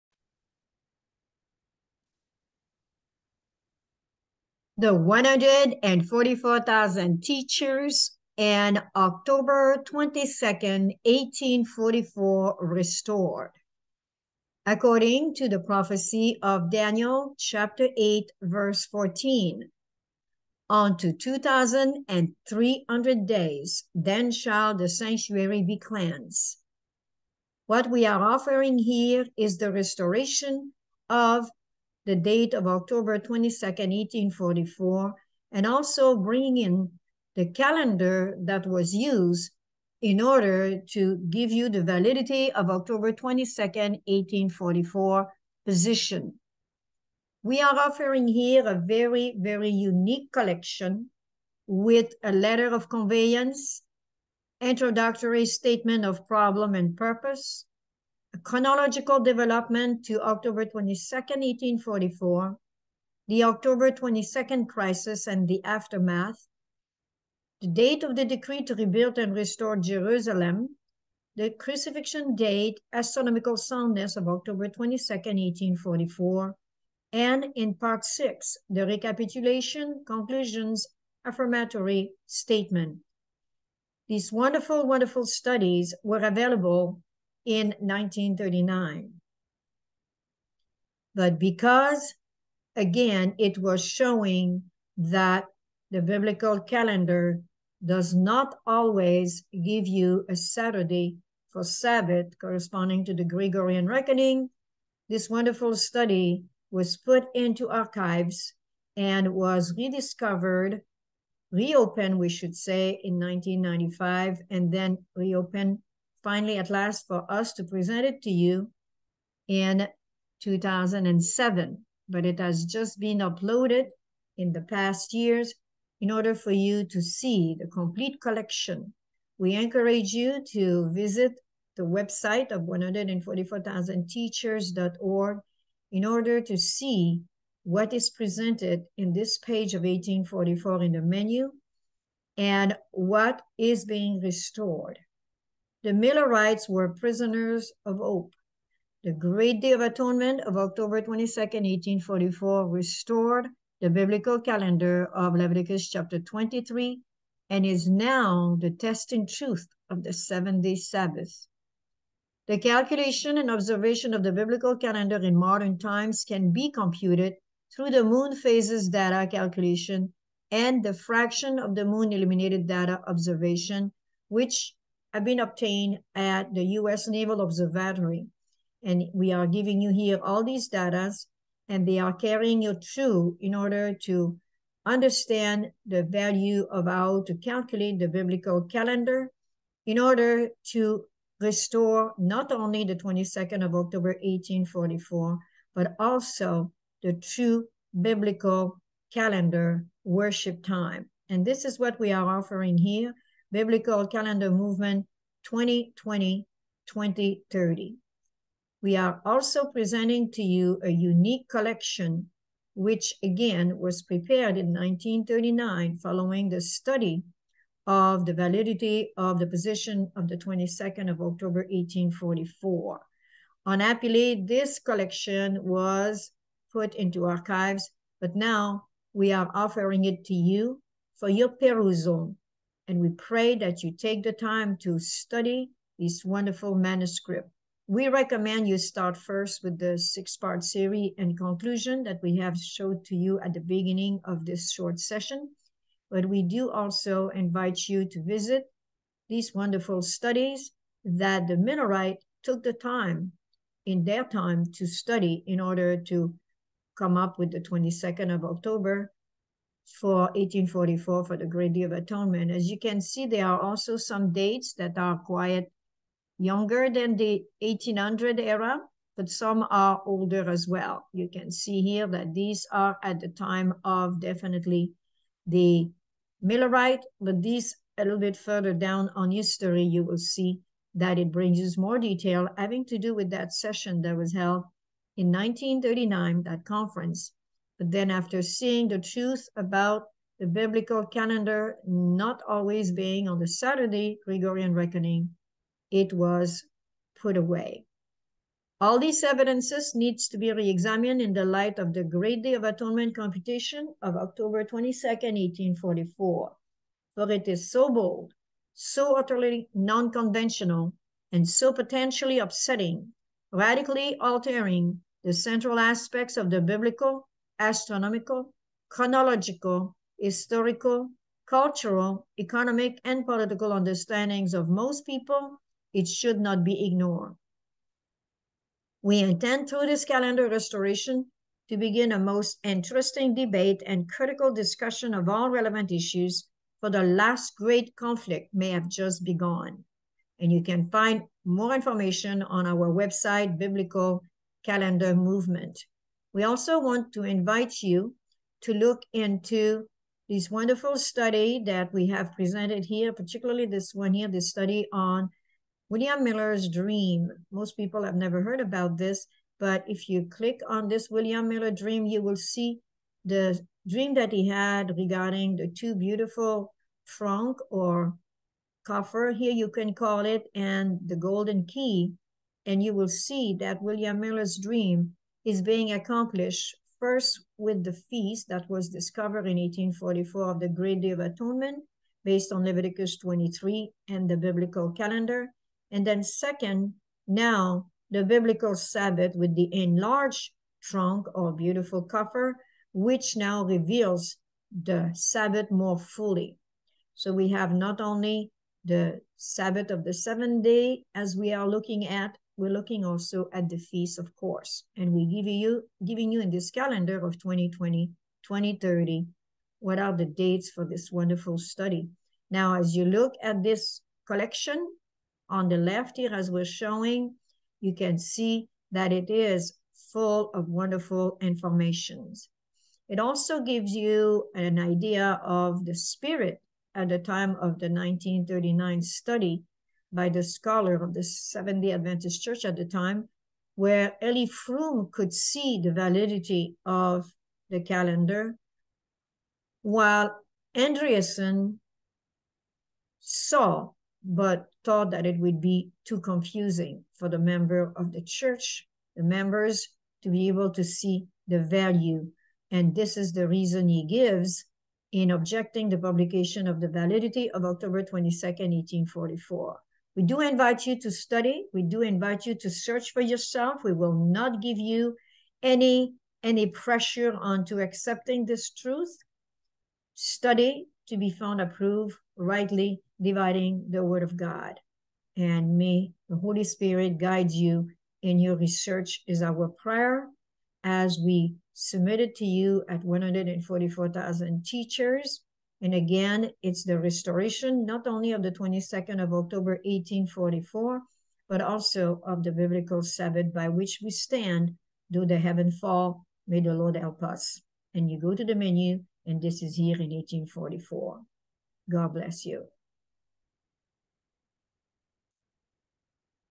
Repairing the Time - 1844
“All these evidences need to be re-examined in the light of the Great Day of Atonement computation of October 22 1844, for it is so bold, so utterly non-conventional, and so potentially upsetting, radically altering the central aspects of the Biblical, Astronomical, Chronological, Historical, Cultural, Economic and Political understanding of most people, it should not be ignored.

We intend through this calendar restoration to begin a most interesting debate and critical discussion of all relevant issues, for the last great conflict may have just begun.” Biblical Calendar Movement